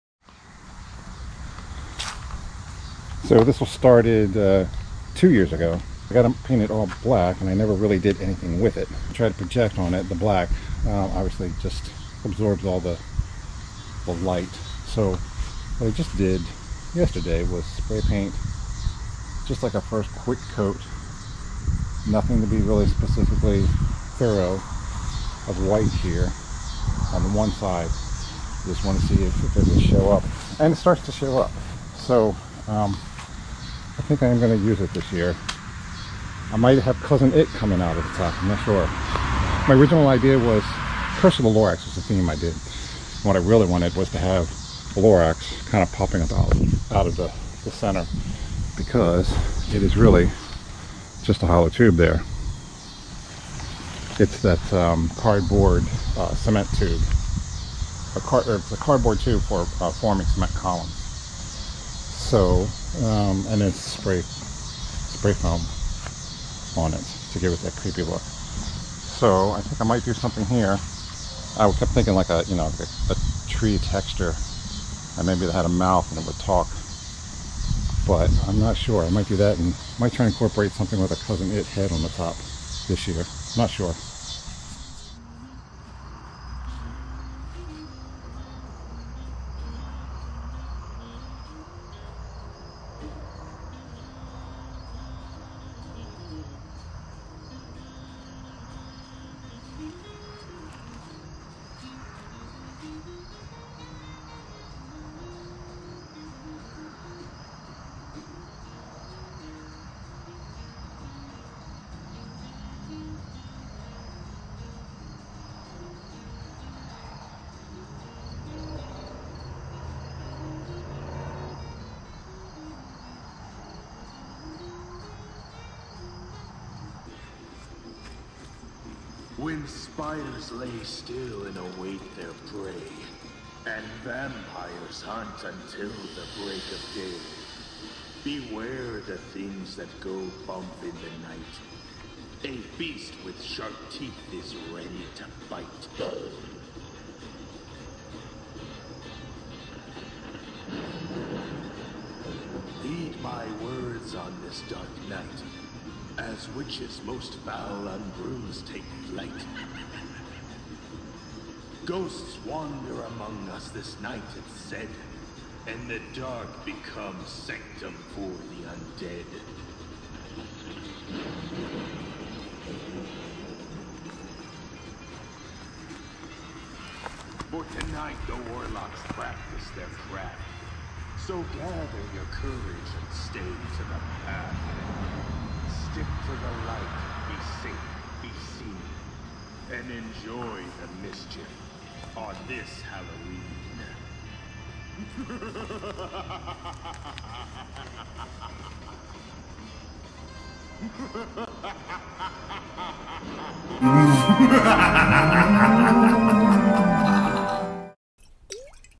tree projection test
I made this spooky tree prop 2 years ago. I intended to feature it in a Curse of the Lorax theme I did, but I ran out of time to properly incorporate it. Last year I never pulled it out of storage. This year, I'm finally getting around to attempting a projection on it. Here are the first explorations with that.